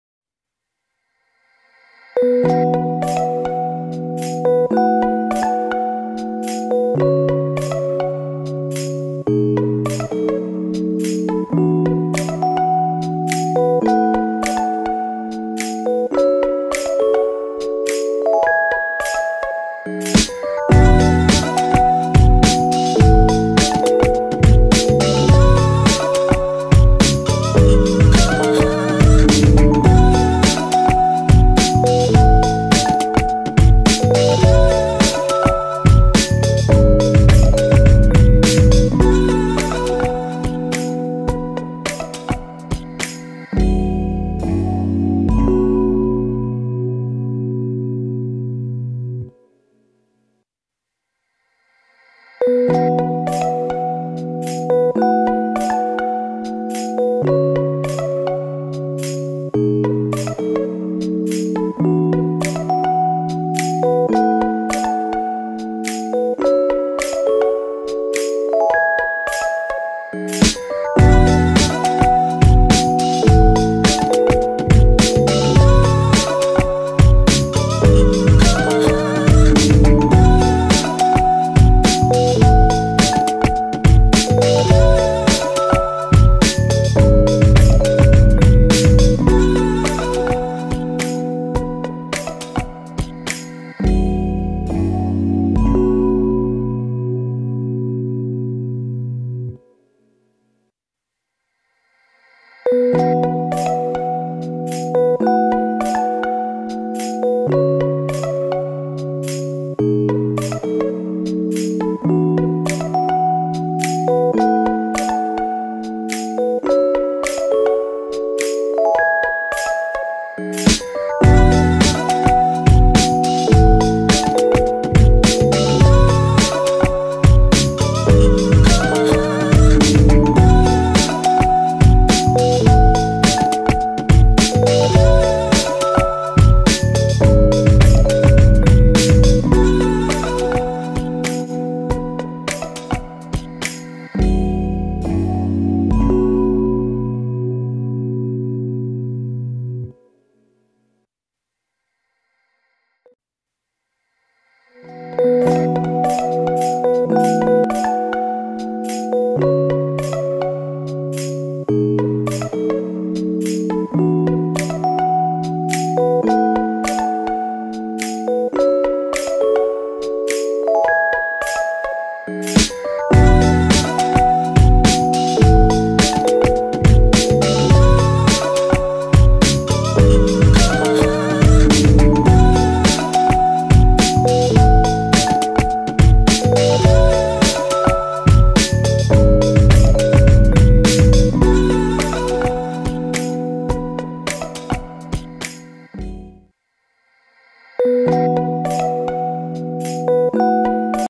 Use of Waste Tharmacol piece, glass bottle & leaf
Desposal plate piece & bottle decorating with light & home decor idea